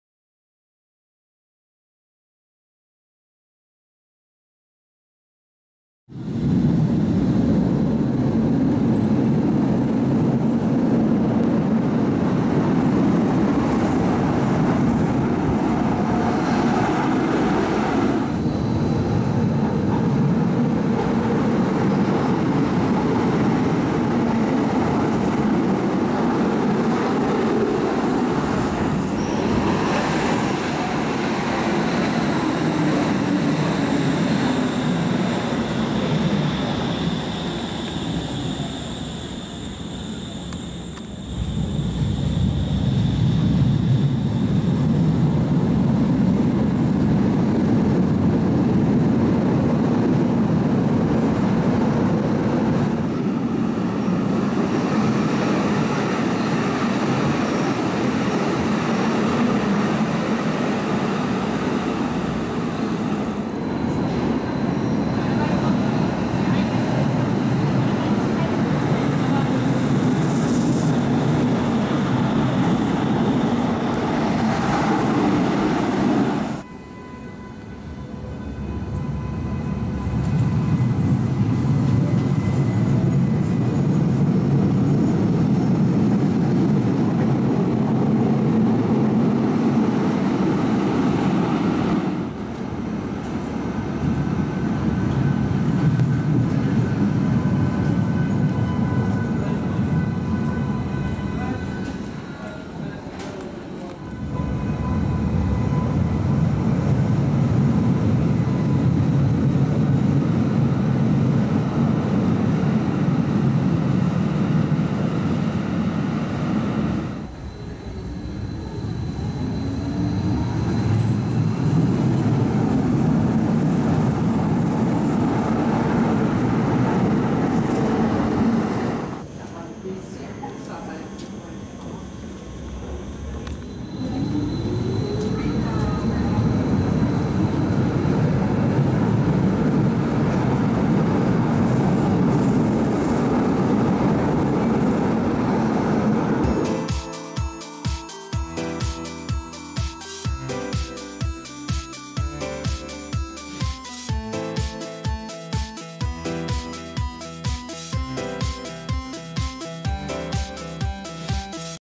Die U-Bahn-Station Hannover Hbf
Im Oktober 2020 konnte ich an der U-Bahn-Station der Üstra am Hbf in Hannover einige U-Bahnen der verschiedensten Baureihen und Linien filmen. Leider konnte ich in der kurzen Zeit nicht so viele Aufnahmen machen, auch genauere Infos über die Baureihen habe ich mir als ortsunkundiger erspart. Ich wünsche euch dennoch viel Spaß mit dem kurzen Video!